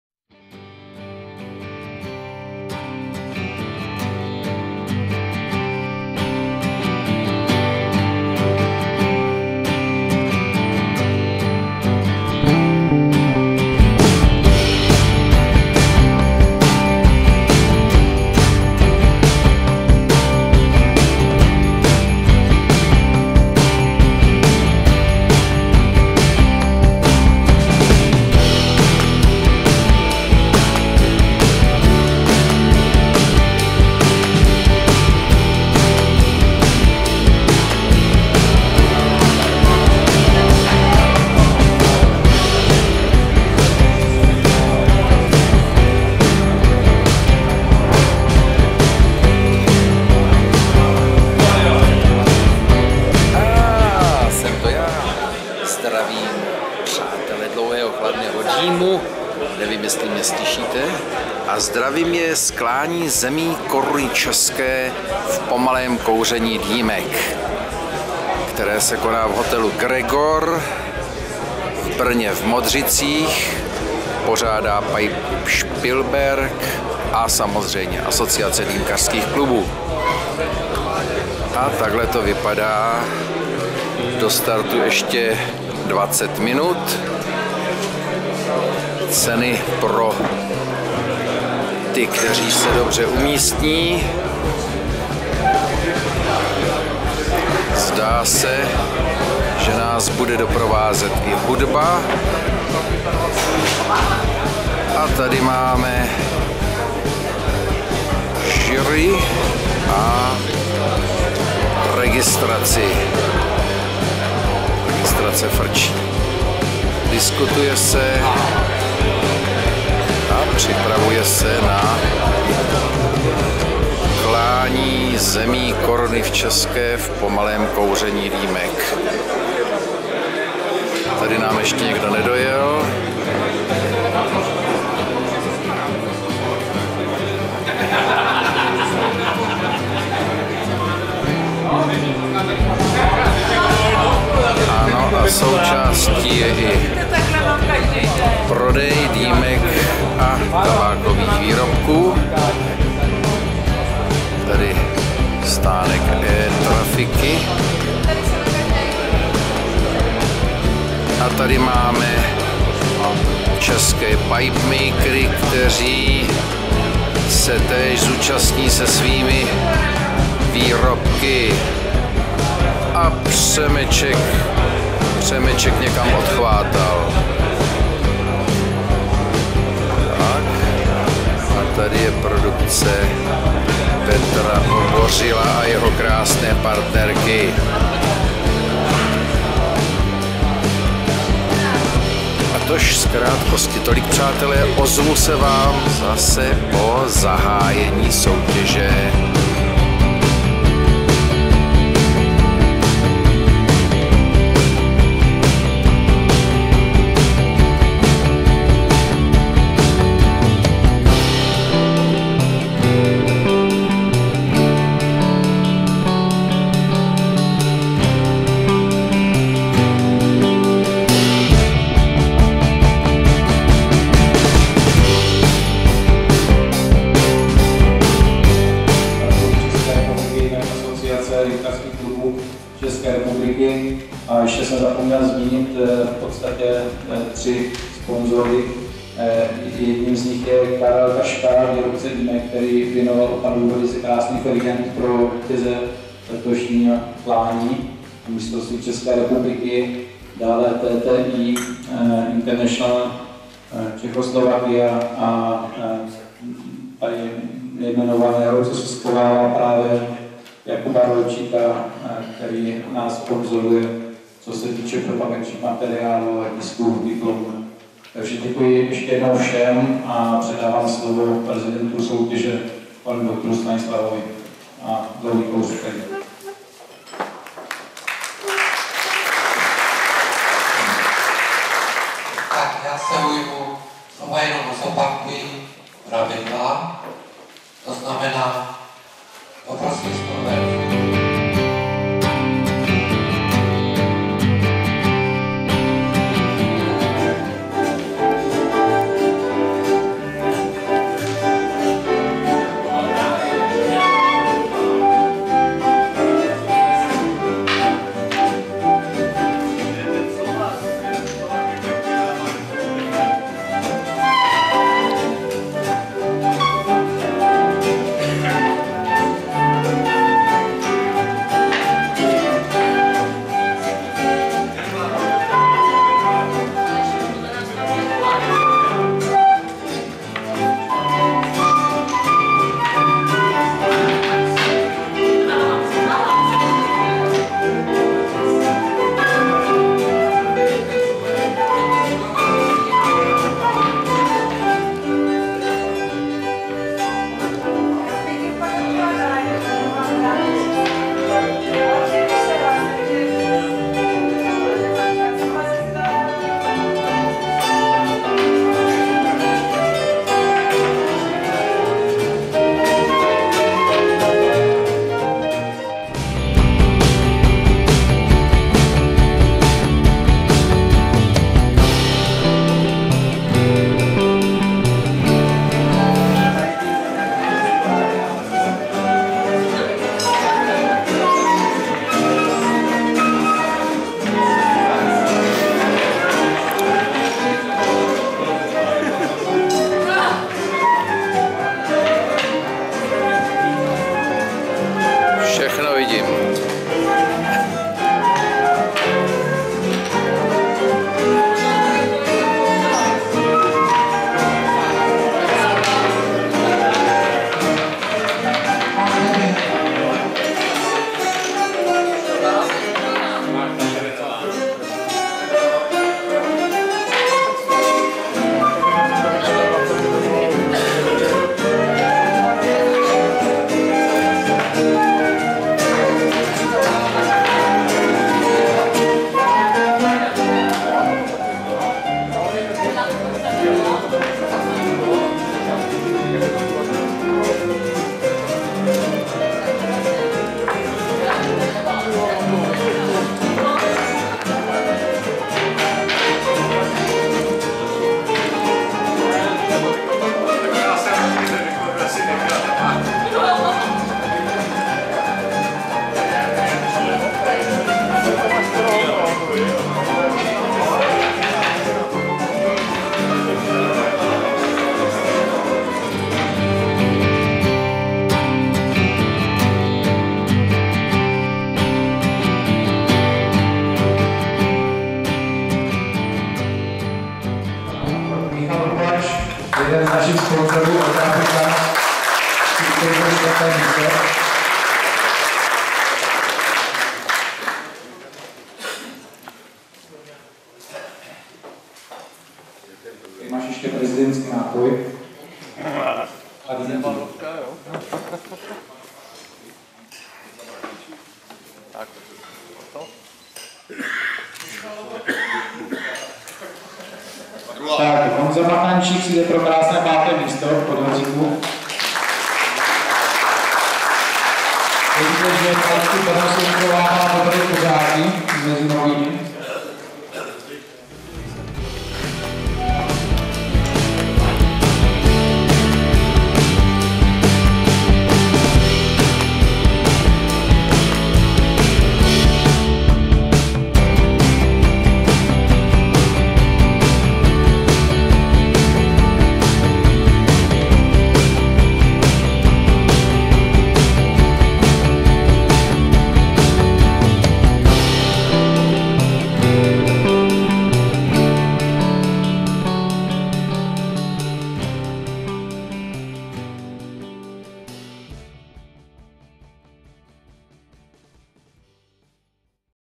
Klání zemí Koruny České v pomalém kouření dýmek a Mistrovství České republiky
V sobotu, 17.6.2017 se konalo XVII. Klání zemí Koruny české a XXV. Mistrovství České republiky v pomalém kouření dýmek v hotelu Gregor v Brně - Modřicích. Pro ty, kteří se zúčastnit nemohli přináším pár záběrů z této akce, aby se alespoň touto cestou tak trochu nadýchali atmosféry z tohoto každoročního setkání a zápolení dýmkařů.